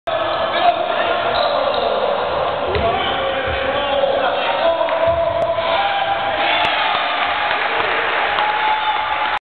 Oscar Perez Hits Open Three-Pointer
San Pedro High Boys Basketball vs. South Gate Highlights (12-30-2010)